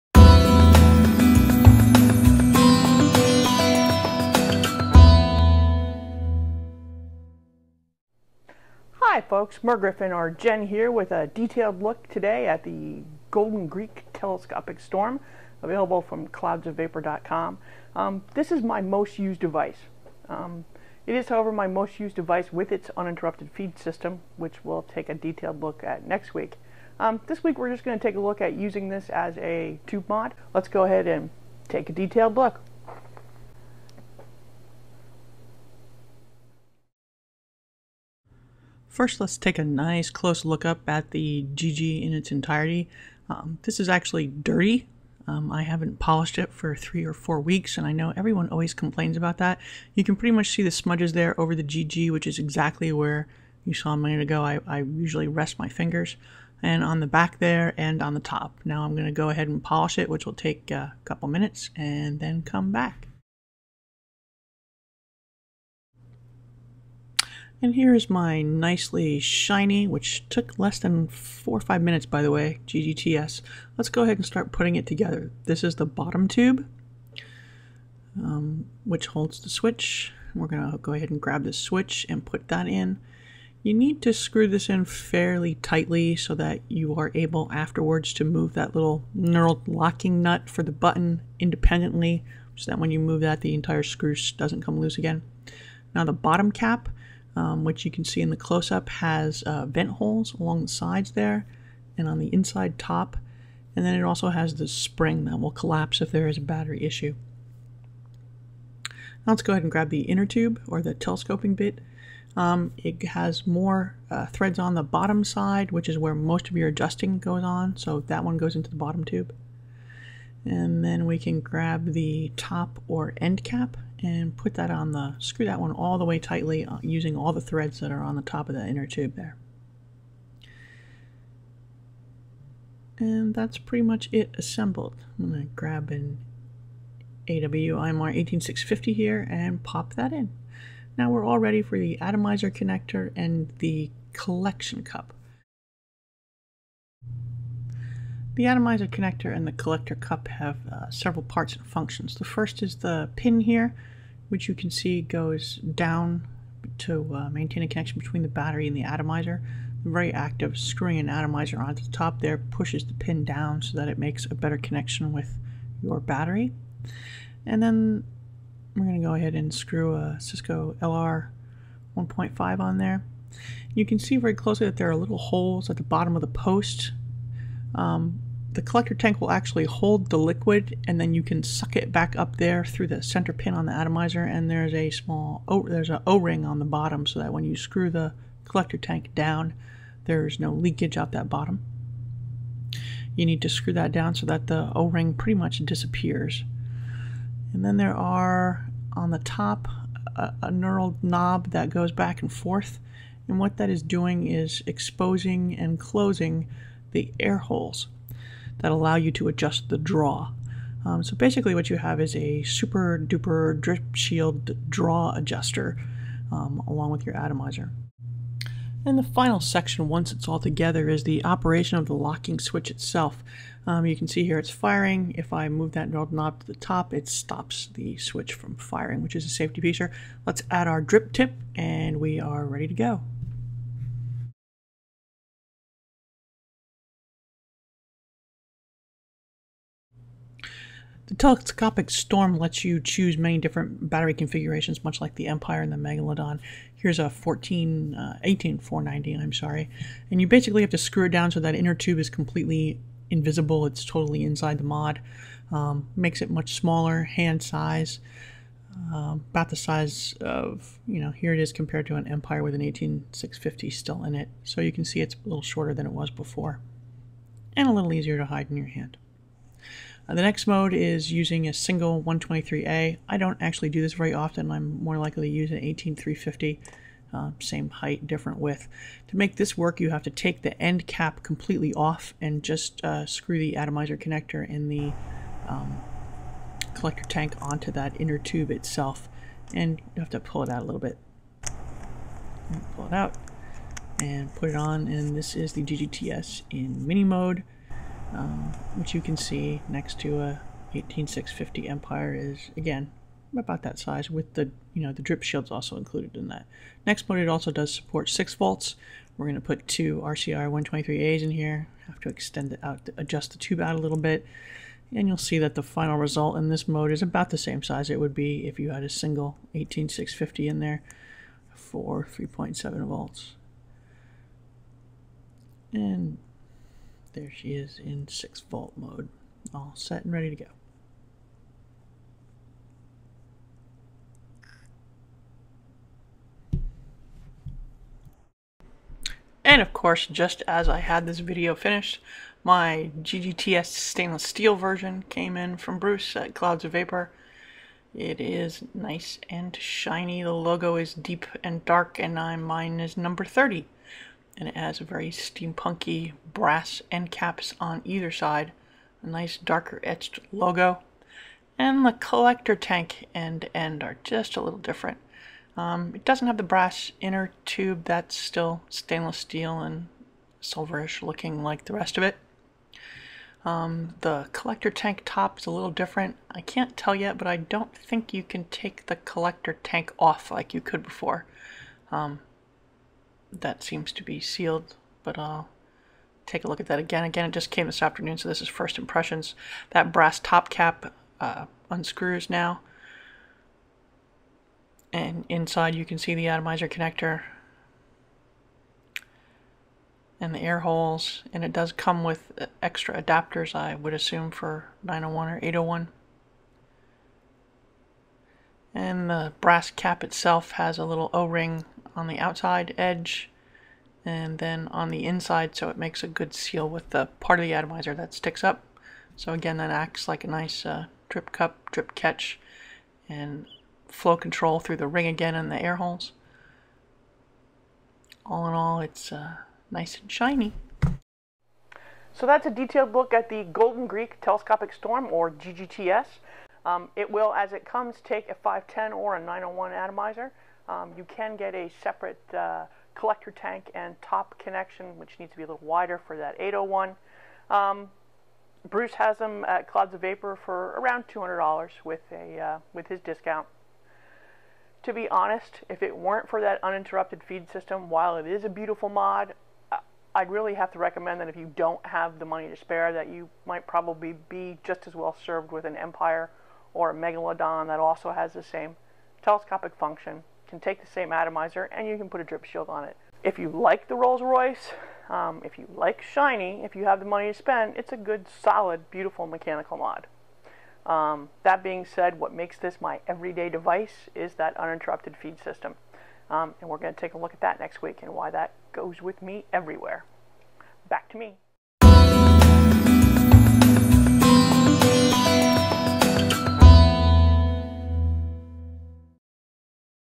Golden Greek Telescopic Storm
Detailed look at the GGTS Nickel Plated and Stainless Steel.

Catch my show, 10pm Friday nights on Vapetv.com